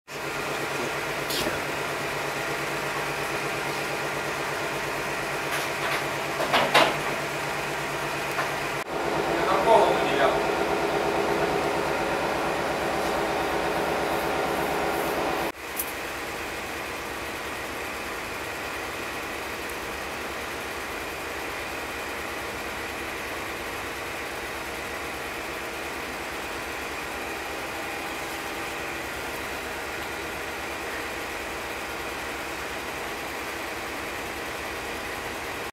О тяжкой доле вахтенного механика.
Видео  о  правилах  несения  вахтенной  службы  на  судах  Вспомогательного  флота.